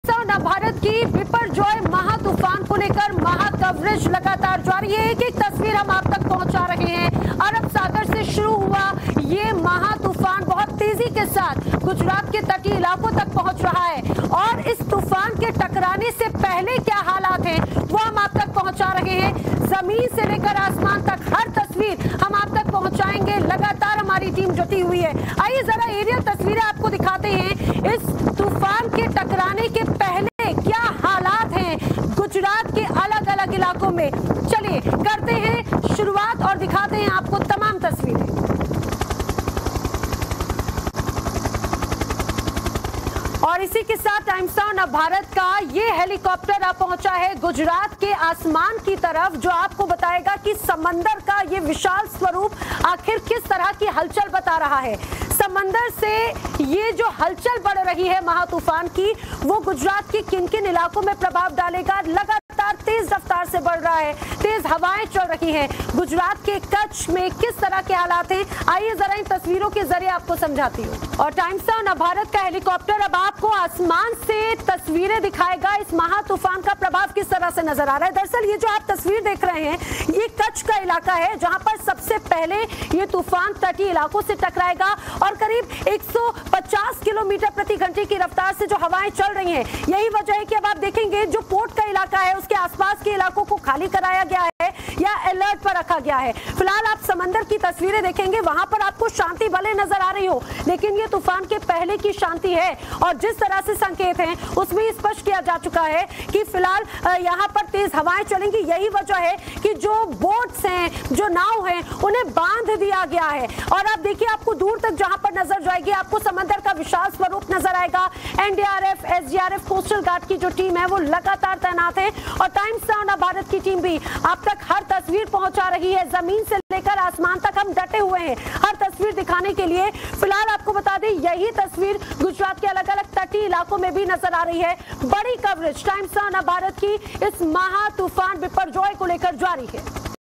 महातूफान Biparjoy से कहां हो सकती है ज्यादा तबाही? देखिए Navbharat की आसमानी रिपोर्ट...
महातूफान Biporjoy (Cyclone Biporjoy) को लेकर देश के कई राज्यों में हाई अलर्ट जारी किया गया है। बता दें कि आज रात महातूफान Biporjoy Gujarat के जखाऊ पोर्ट से टकराएगा।जिसको लेकर तटीय इलाकों को भी खाली करवा दिया गया है। Times Now Navbharat पर समझिए तूफान से कितनी हो सकती है तबाही.